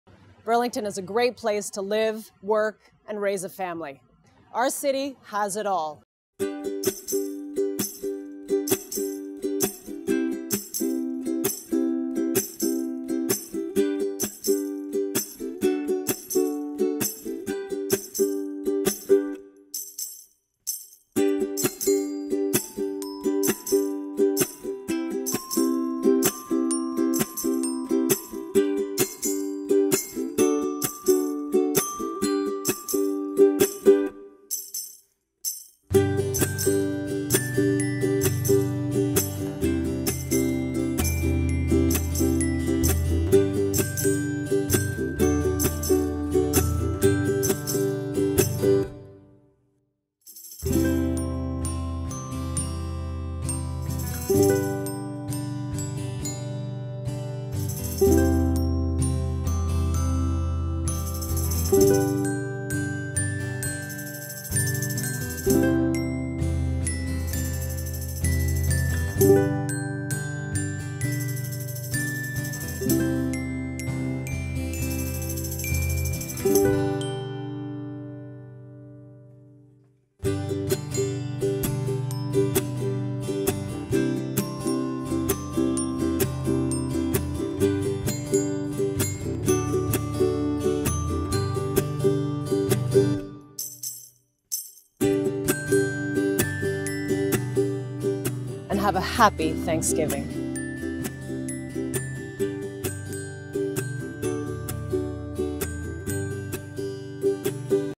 Burlington. Is.
I'm grateful to live, work and raise my family in Burlington. Watch the "Burlington. Is." video, find out if it includes your favourite parts of our city - urban, rural, waterfront, a great downtown, historic neighbourhoods, and much more.